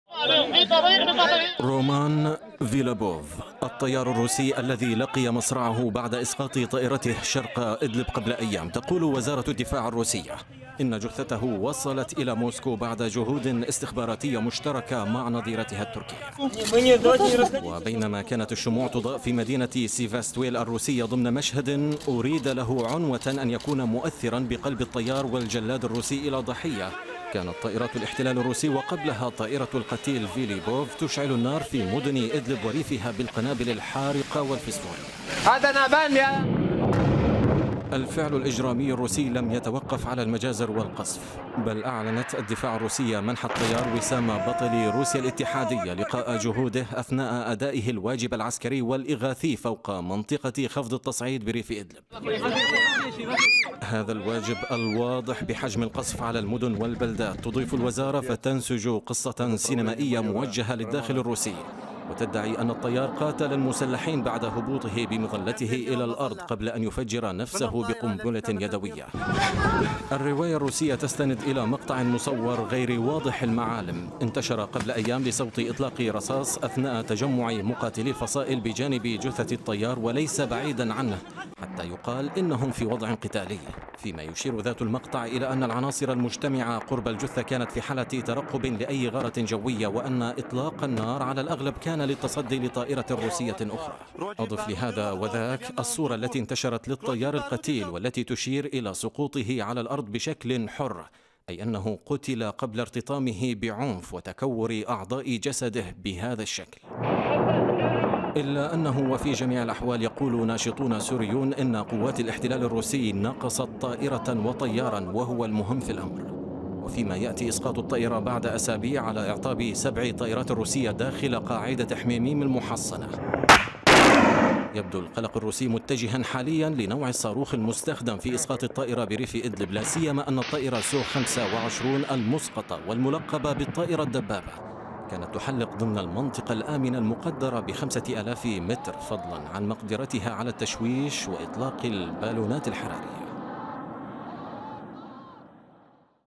روسيا تستعيد جثة طيارها وتنشر قصة "سينمائية" حتى بعد مقتله..ومخاوف من صواريخ جديدة بيد الثوار
CAUTION! - this video contains footages which might NOT be suitable for all audience

قالت وزارة ُالدفاع الروسية إن استخباراتِها نجحت بمساعدةٍ من نظيرتِها التركية، في استعادةِ جثمانِ الطيار الروسي رومان فيليبوف الذي أُسقطت طائرتُه بريفِ إدلب أثناءَ غاراتٍ كان يُنفذُها على مدينةِ سراقب قبل ثلاثة أيام .